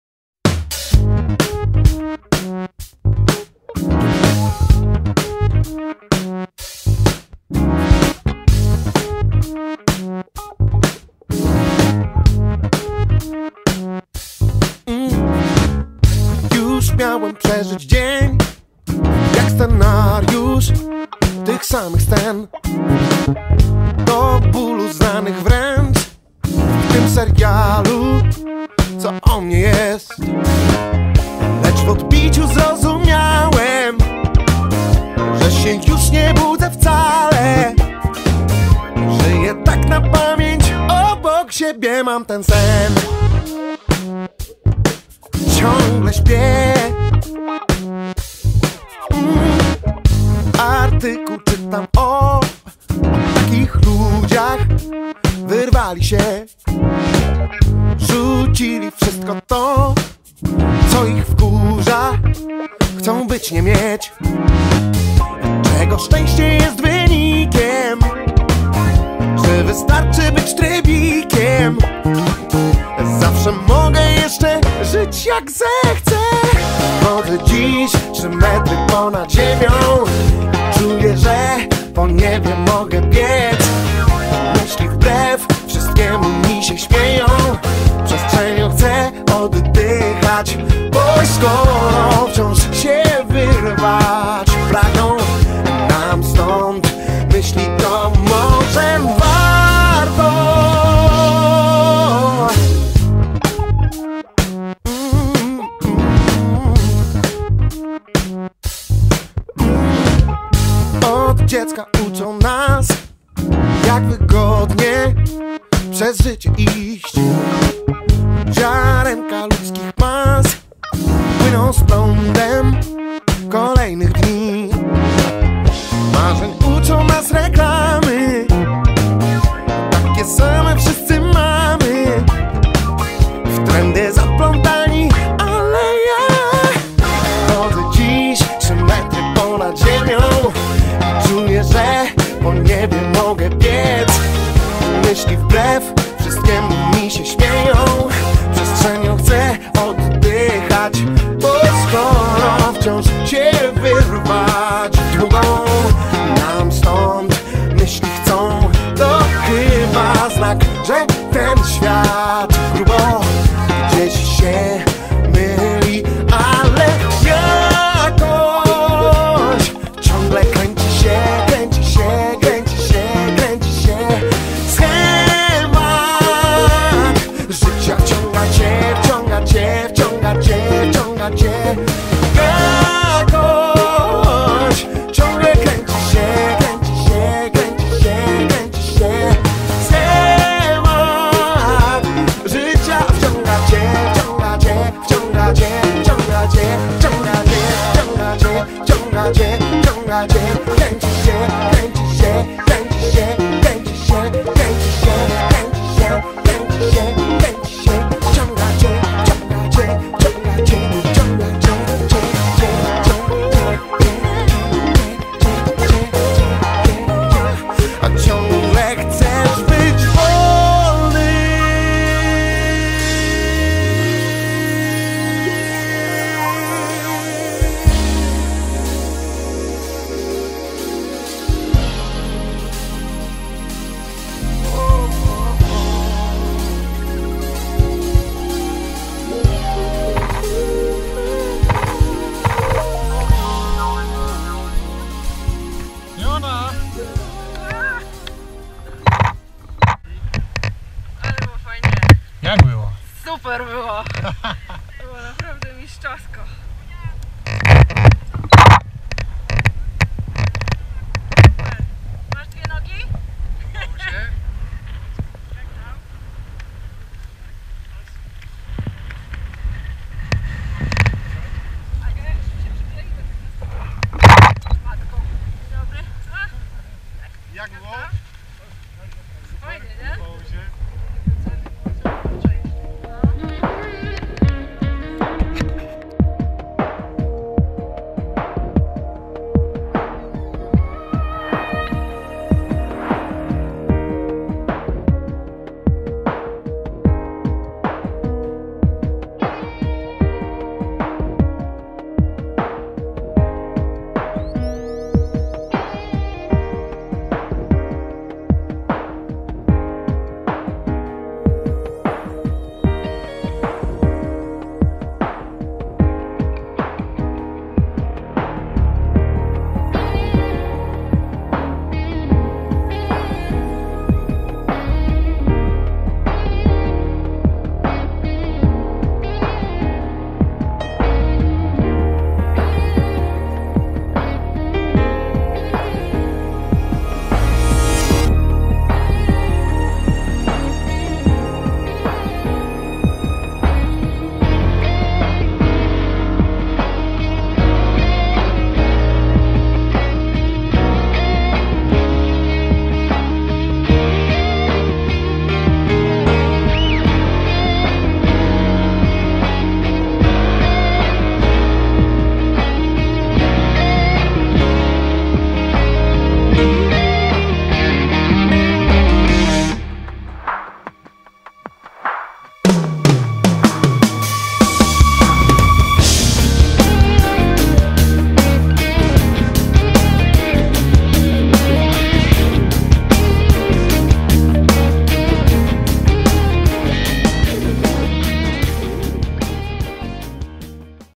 Magda Tandem Chrcynno 11.09.2015
Skok z 4200m npm w tandemie, lotnisko Aeroklubu Warszawskiego, Chrcynno SkyDive. Magda.